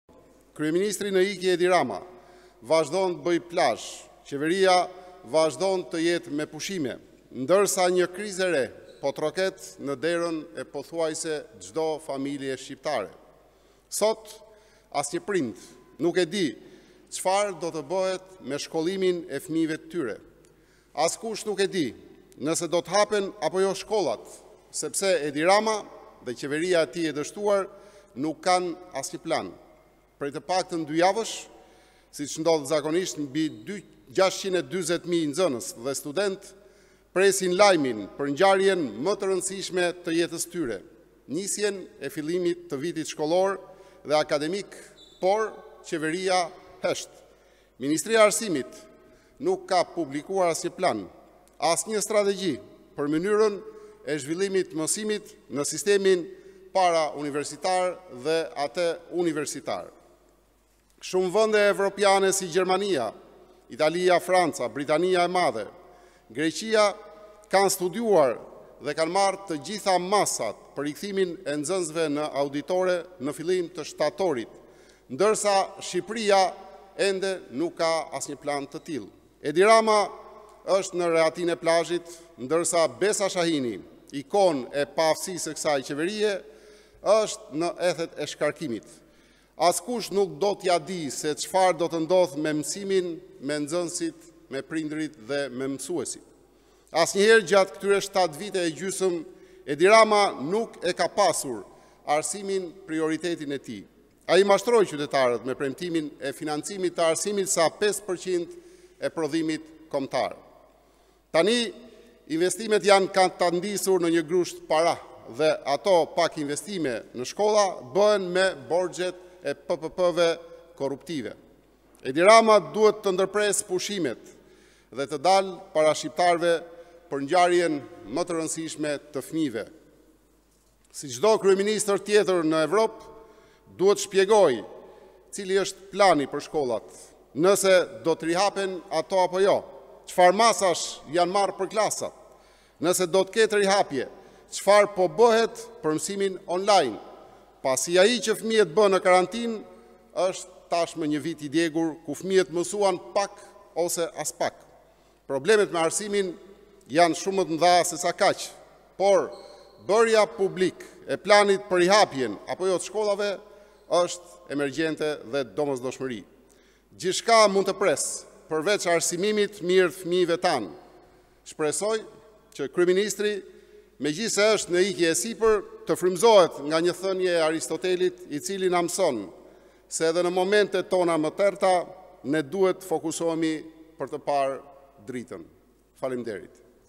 Report TV - PD: Qeveria duhet të lërë pushimet dhe të merret me planin për rihapjen e shkollave
Ish- deputeti i Partisë Demokratike Luçiano Boçi, mes akuzave për dështimet e kësaj qeverie, bën thirje për  bërjen e një plani strategjik për hapjen e shkollave nga ana e qeverisë.

"Prej të paktën dy javësh, siç ndodh zakonisht, mbi 640 mijë nxënës dhe studentë presin lajmin për ngjarjen më të rëndësishme të jetës së tyre, nisjen e fillimit të vitit shkollor dhe akademik, por qeveria hesht. Problemet me arsimin janë shumë më të mëdha sesa kaq, por bërja publik e planit për rihapjen apo jo të shkollave është emergjent. Ministria e Arsimit nuk ka publikuar asnjë plan, asnjë strategji për mënyrën e zhvillimit të mësimit në sistemin parauniversitar dhe atë universitar. Shumë vende evropiane si Gjermania, Italia, Franca, Britania e Madhe, Greqia kanë studiuar dhe kanë marrë të gjitha masat për rikthimin e nxënësve në auditorë në fillim të shtatorit, ndërsa Shqipëria nuk ka asnjë plan të tillë." Shprehet BoçiAi bën thirje që kryeministri Rama duhet të lerë pushimet dhe ti japë një zgjidhje mirja nxënësve dhe studentëvë të cilët janë në pritje të hapjes së shkollave:

"Shqipëria ka një problem të madh qe prodhon krizë pas krize. Ky problem është qeveria e paaftë, e papaërgjegjshme dhe e korruptuar që drejton vendin.